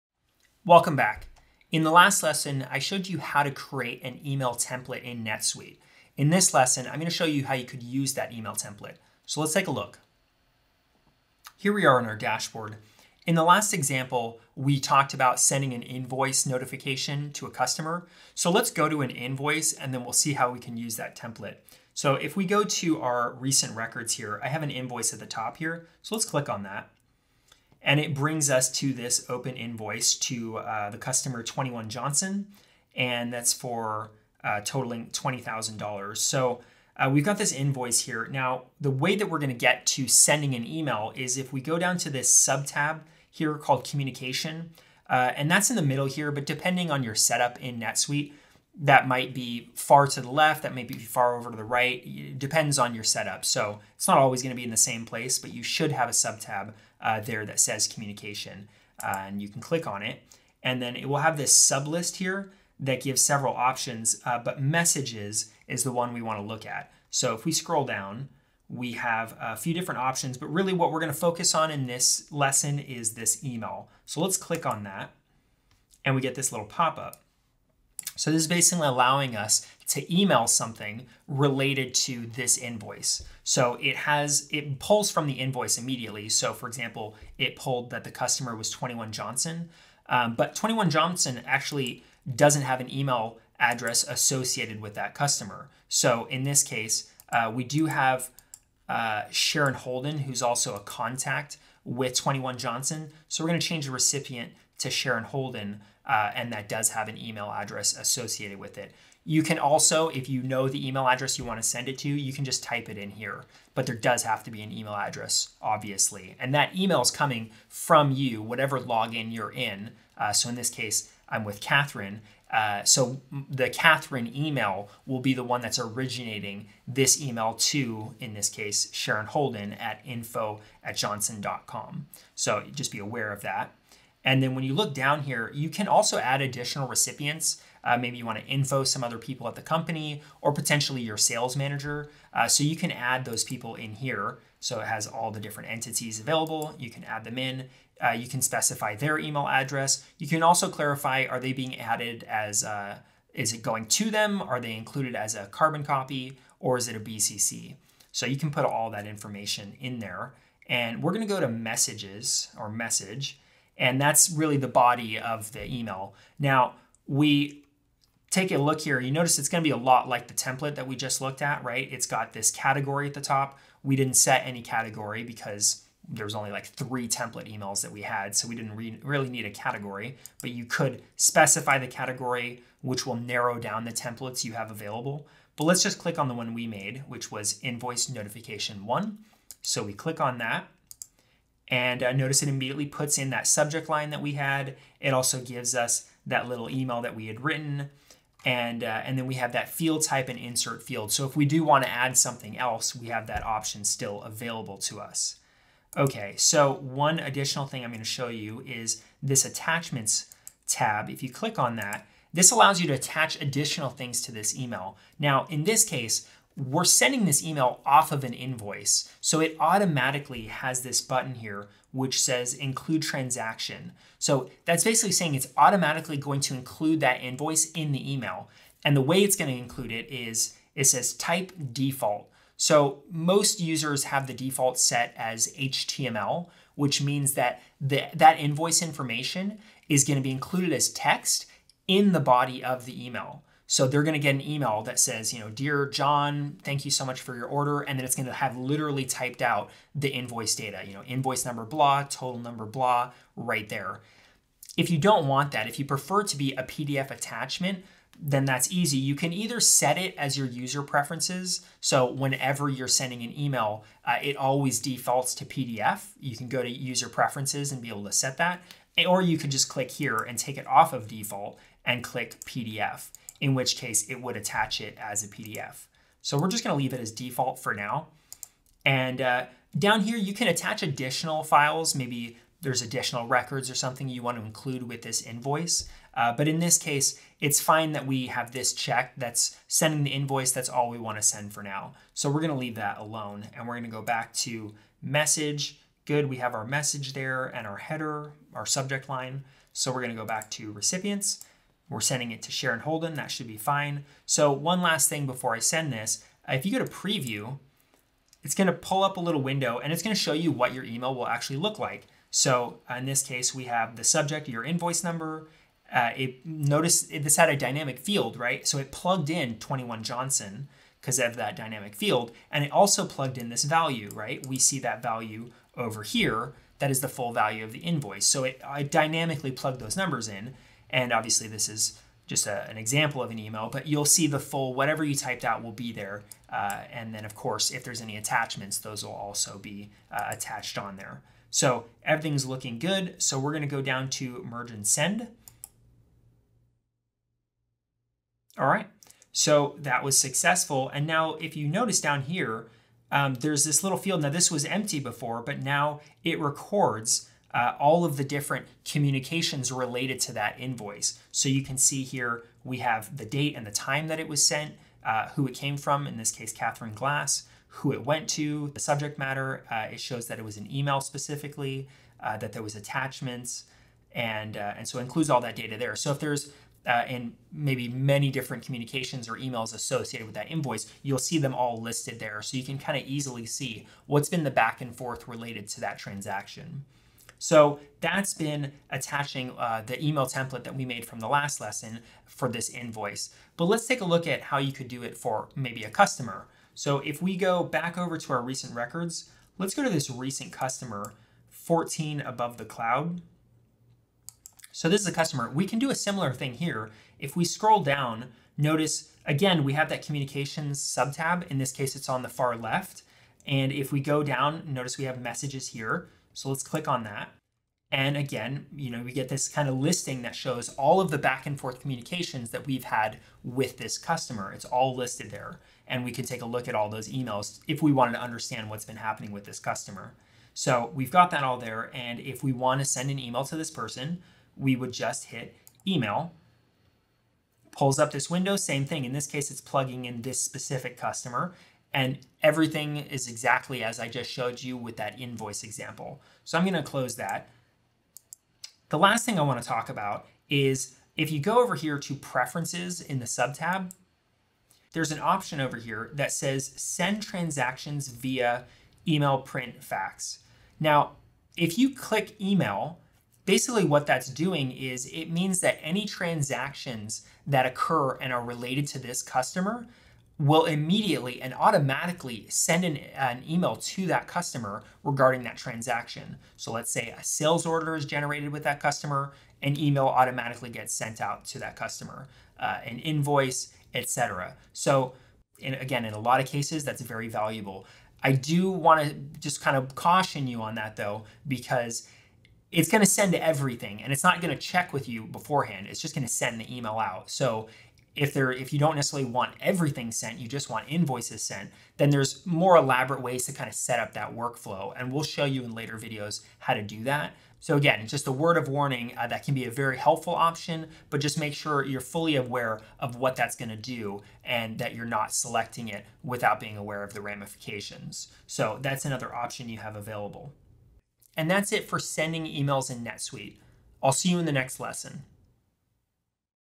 NetSuite How to Send Emails
In this video, we show you how to send emails and email templates from records in NetSuite.

These videos are part of a series of NetSuite courses, which you can access

Intro - 00:00
Record Emails - 00:11
Creating Transaction Email - 01:18
Email Body - 02:42
Email Attachments - 03:37
Sending Email - 04:55
Email Log - 05:47
Customer Email - 06:30
Customer Email Preferences - 07:30
Outro - 08:40